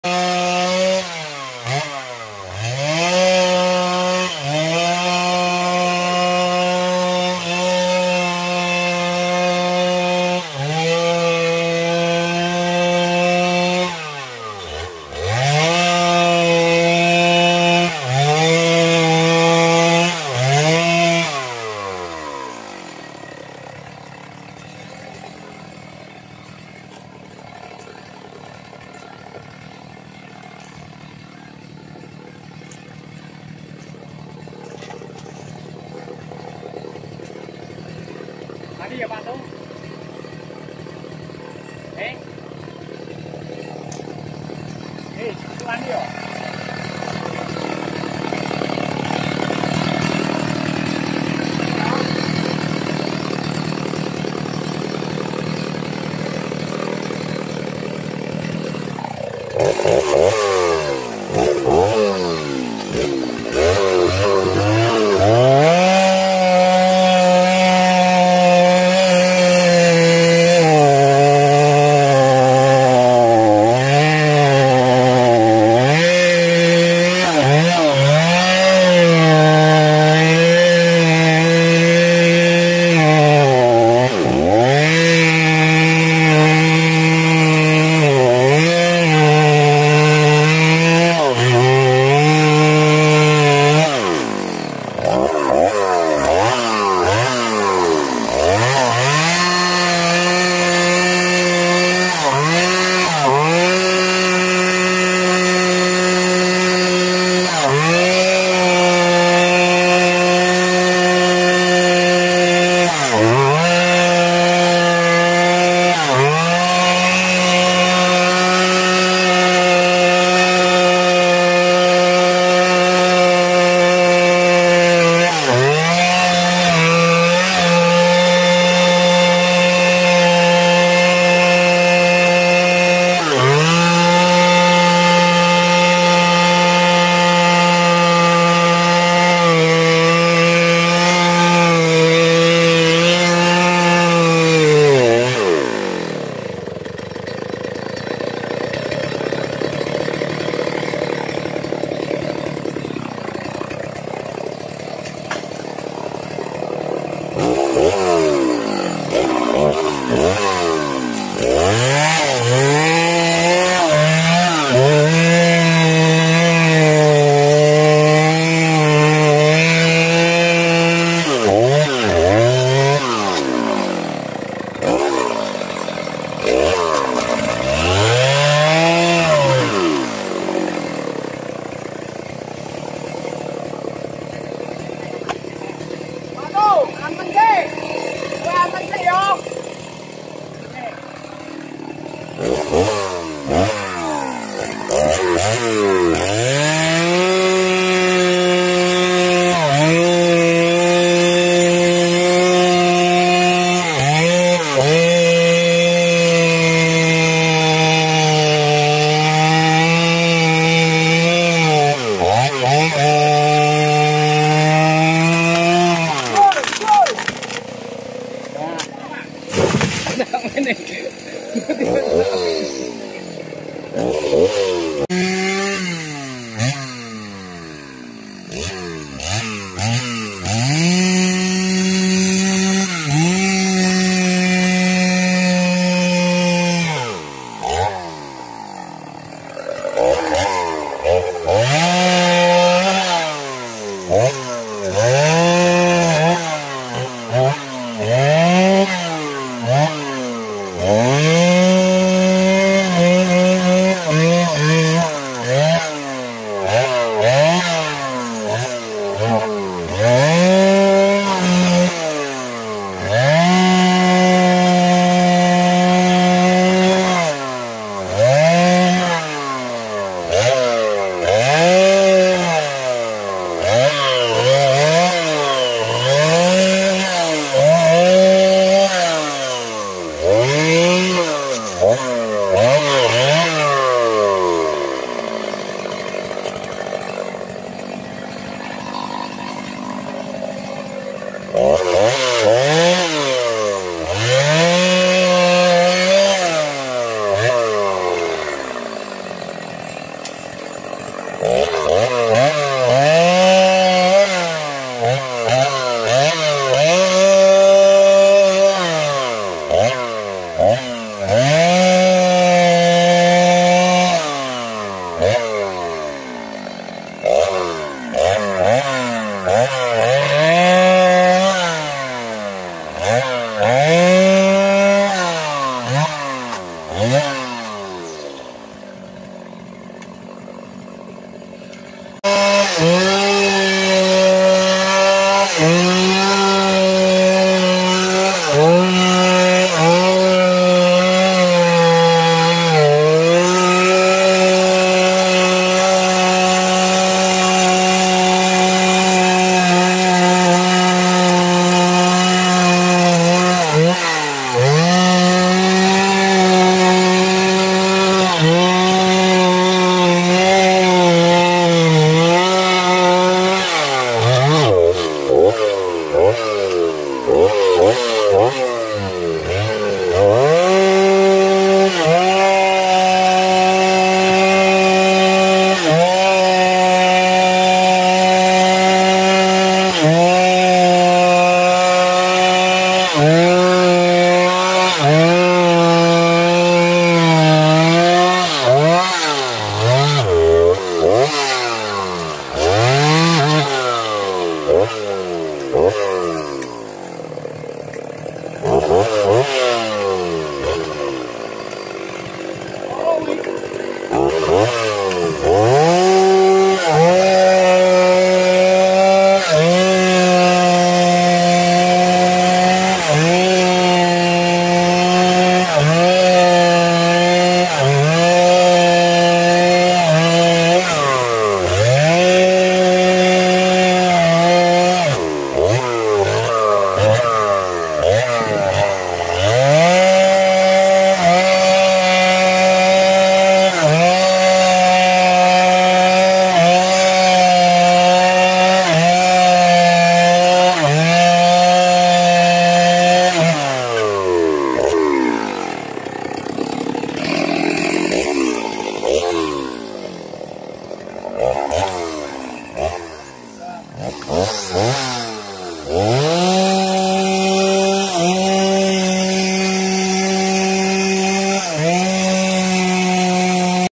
🔴AKIBAT MERESAHKAN WARGA ‼️Tebang Mahoni sebelah kandang ternak Ayam ( Part 1 )
🔴AKIBAT MERESAHKAN WARGA ‼️Tebang Mahoni sebelah kandang ternak Ayam

Penebangan pohon mahoni yang ada di sekitar rumah warga, terutama di sebelah kandang ayam warga yang meresahkan dimana ditakutkan ambruk mengarah ke kandang ayam saat terkena angin lepas.